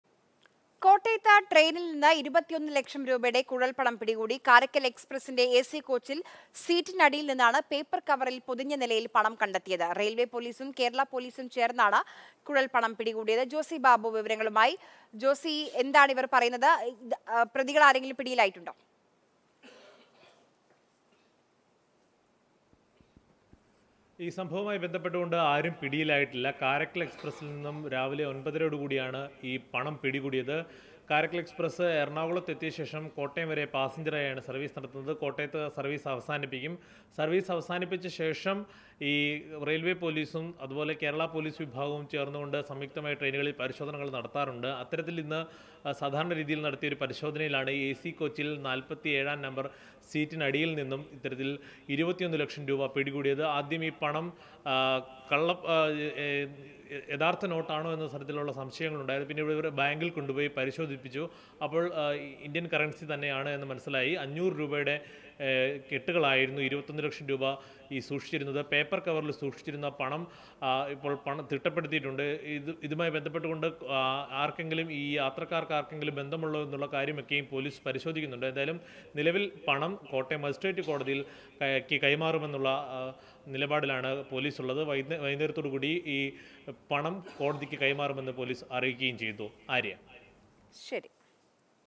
കോട്ടയത്ത് ട്രെയിനിൽ നിന്ന് 21 ലക്ഷം രൂപയുടെ കുഴൽപണം പിടികൂടി | Mathrubhumi News
കാരയ്ക്കൽ എക്സ്പ്രസിന്റെ എസി കോച്ചിൽ സീറ്റിനടിയിൽ നിന്നാണ് പേപ്പർകവറിൽ പൊതിഞ്ഞനിലയിൽ പണം കണ്ടെത്തിയത്. റെയിൽവേ പോലീസും കേരളാ പോലീസും ചേർന്നാണ് കുഴൽപണം പിടിച്ചത്.

Happy viewing!